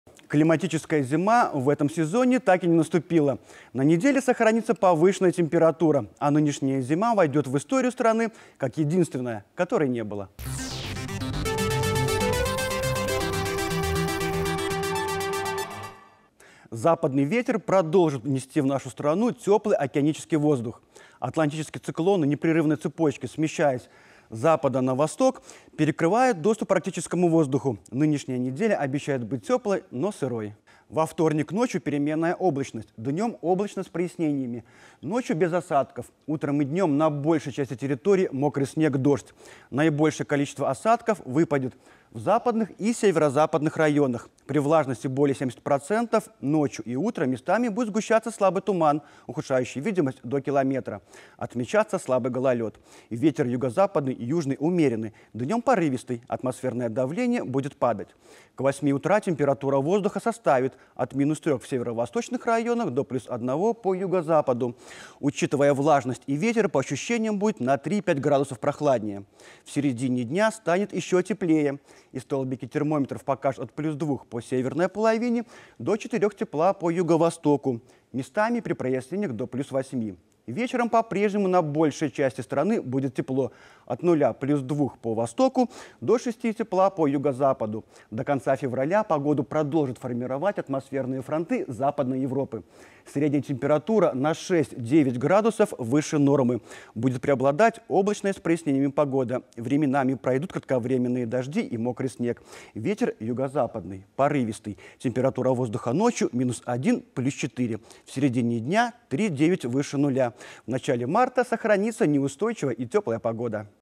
Прогноз на 25 февраля: зима, похоже, закончилась не начавшись
Климатическая зима в этом сезоне так и не наступила. На неделе сохранится повышенная температура. А нынешняя зима войдет в историю страны как единственная, которой не было.
Западный ветер продолжит нести в нашу страну  теплый океанический воздух. Атлантические циклоны, непрерывной цепочкой смещаясь с запада на восток, перекрывают доступ арктическому воздуху. Нынешняя неделя обещает быть теплой, но сырой. Во вторник ночью переменная облачность, днем облачно с прояснениями. Ночью без осадков, утром и днем на большей части территории мокрый снег, дождь – наибольшее количество осадков выпадет в западных и северо-западных районах. При влажности более 70%, ночью и утром местами сгуститься слабый туман, удушающий видимость до километра, отмечаться слабый гололед. Ветер юго-западный, южный умеренный, днем порывистый. Атмосферное давление будет падать.